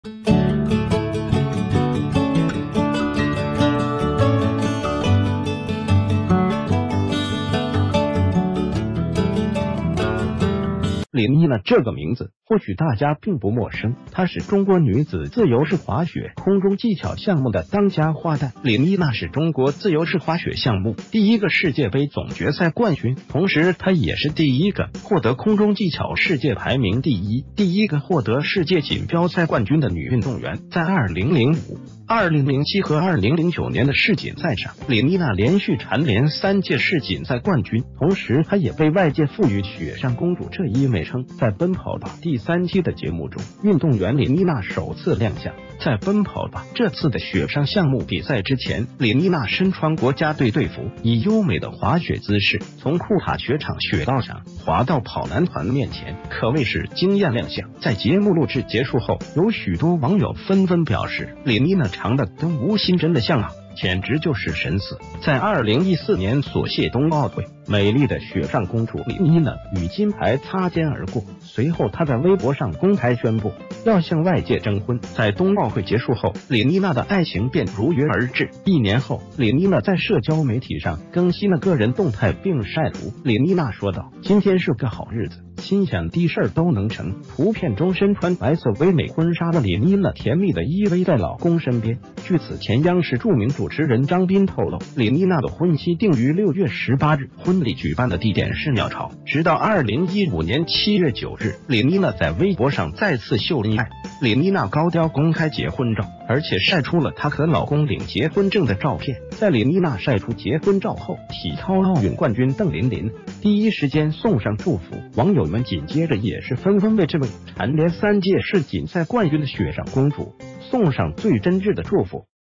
曾在世錦賽奪得三連冠，長相神似吳昕，老公卻大她14歲！
谢谢！
    李妮娜這個名字或許大家并不陌生，她是中國女子自由式滑雪空中技巧項目的當家花旦，李妮娜是中國自由式滑雪項目第一個世界杯總決賽冠軍，同時她也是第一個獲得空中技巧世界排名第一，第一個獲得世界錦標賽冠軍的女運動員。在2005、2007和2009年的世錦賽上，李妮娜連續蟬聯三屆世錦賽冠軍，同時她也被外界賦予“雪上公主”這一美稱。
    在《奔跑吧》第三期的節目中，運動員李妮娜首次亮相，在《奔跑吧》這次的雪上項目比賽之前，李妮娜身穿國家隊隊服，以優美的滑雪姿勢，從庫塔雪場雪道上滑到跑男團面前，可謂是驚艷亮相。在節目錄制結束后，有許多網友紛紛表示：“李妮娜長的跟吳昕真的像啊！簡直就是神似！”
    在2014年索契冬奧會，美麗的雪上公主李妮娜與金牌擦肩而過。隨后她在微博上公開宣布要向外界征婚。在冬奧會結束后，李妮娜的愛情便如約而至。一年后，李妮娜在社交媒體上更新了個人動態并曬圖，李妮娜說道：“今天是個好日子，心想滴事兒都能成！”圖片中身穿白色唯美婚紗的李妮娜甜蜜的依偎在老公身邊。據此前央視著名主持人張斌透漏：“李妮娜的婚期定于6月18日，婚禮舉辦的地點是鳥巢。
    直到2015年7月9日，李妮娜在微博上再次“秀恩愛”，李妮娜高調公開結婚照，而且曬出了她和老公領結婚證的照片。在李妮娜曬出結婚照后，體操奧運冠軍鄧琳琳第一時間送上祝福，網友們緊接著也是紛紛為這位蟬聯三屆世錦賽冠軍的“雪上公主”送上最真摯地祝福。
    對于這位三屆世錦賽冠軍和兩屆冬奧會亞軍神秘而又快速的愛情，她的許多粉絲都在對她的老公進行打探。據透漏：“李妮娜的老公是北京一家主營“旅游超市”概念的國際旅行社的創始人也是股東之一，結婚時李妮娜33歲，她老公47歲，整整比她大了14歲。”不管如何，都希望這位曾為國家體育項目奪得多次金牌的世錦賽冠軍在以后的生活中可以幸福美滿。